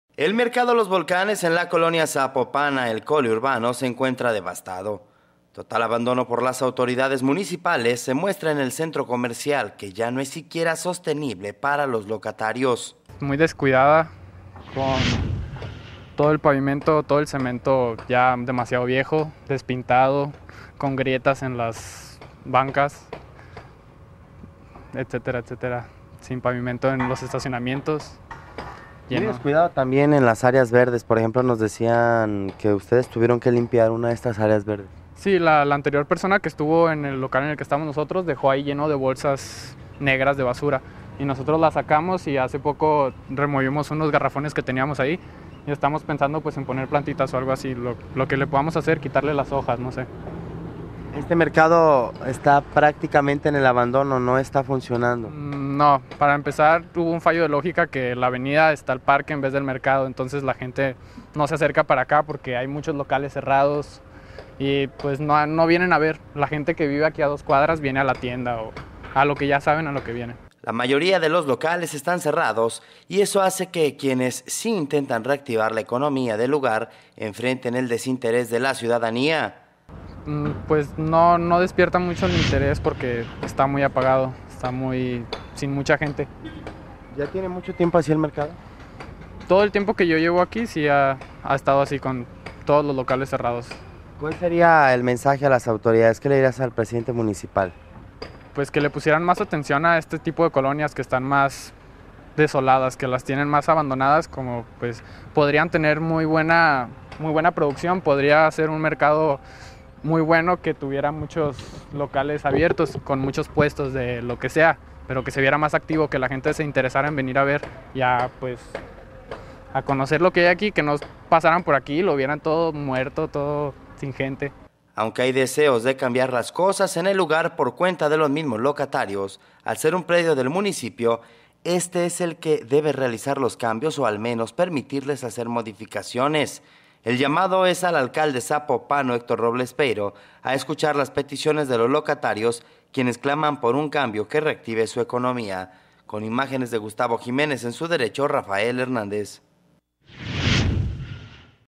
En Su Derecho: Mercado Los Volcanes
Las investigaciones más recientes por parte de la Universidad de Guadalajara señalan que los mercados municipales que se ubican dentro de la zona metropolitana generan alrededor de 70 mil empleos directos, y constituyen una excelente opción para abastecer de  productos básicos a la población, con precios más baratos y alimentos de mayor frescura. Sin embargo, hay mercados como el de Los Volcanes, en Zapopan, que se encuentran en el olvido.